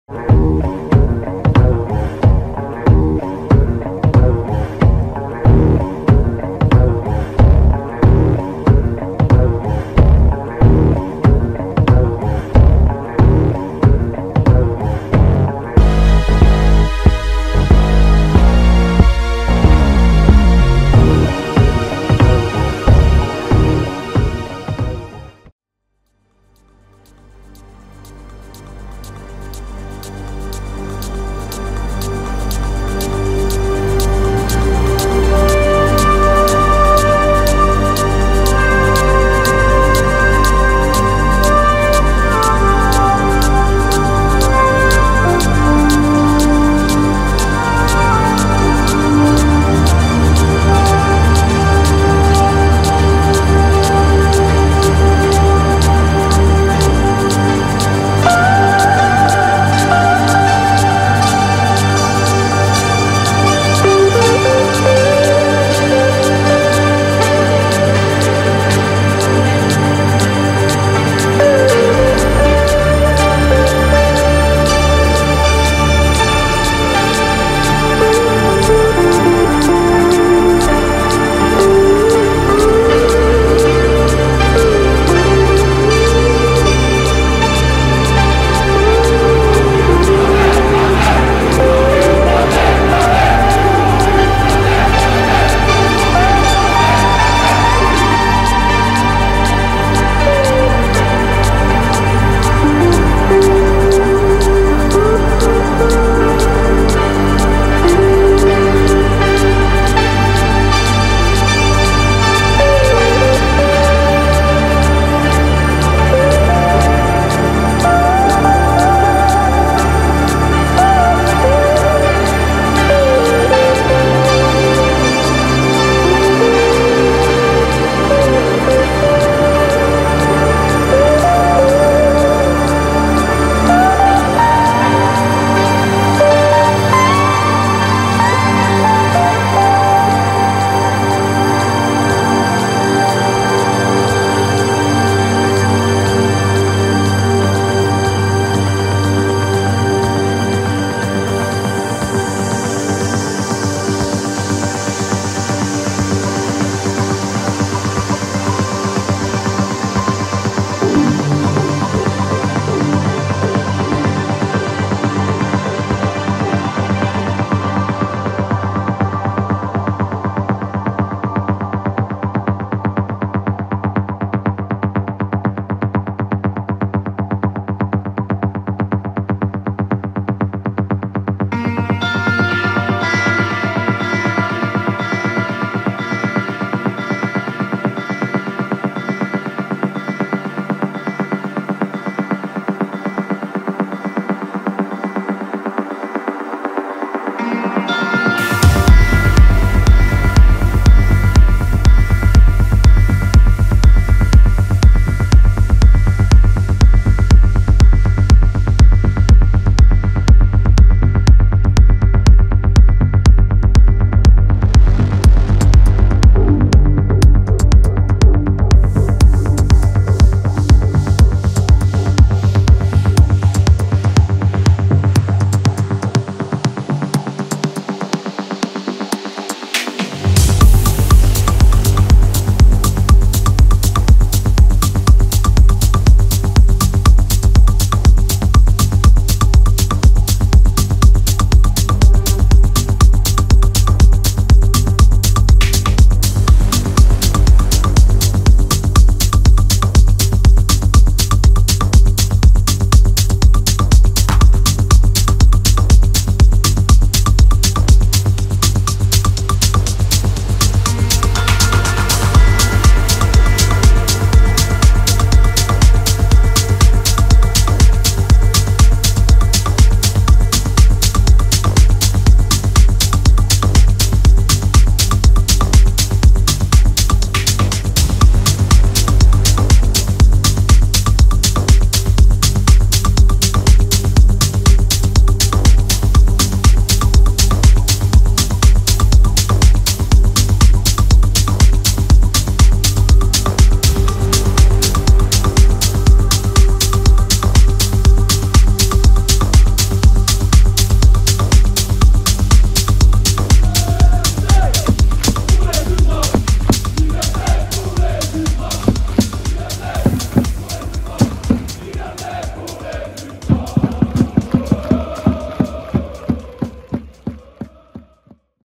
FCN - Lens (03/02/2024) - Brigade Loire
Retour sur les animations et l'action ANS (Association Nationale des Supporters) lors du match opposant le FC Nantes au RC Lens le samedi 3 février 2024.